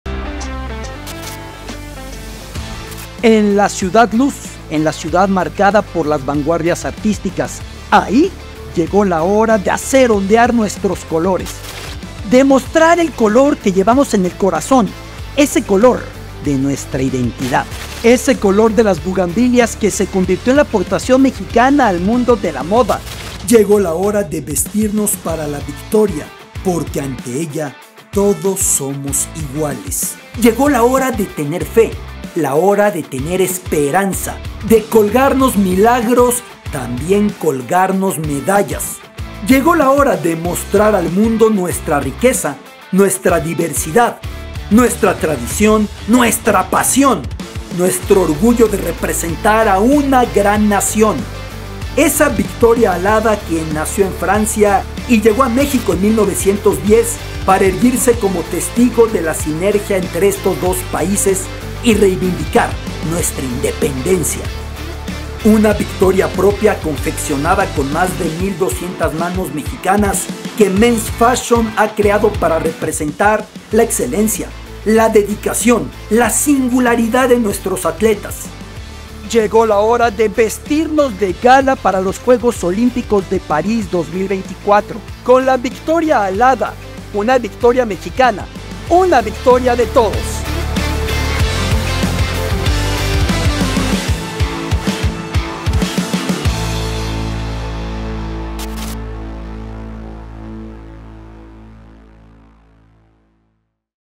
El uniforme olímpico mexicano para París 2024 | Alberto Lati
Alberto Lati nos narra más de lo que representa la “Victoria Alada”, el uniforme que Men's Fashion creó para los deportistas que irán a los Juegos Olímpicos París 2024. Nos llena de orgullo acompañarlos en este viaje.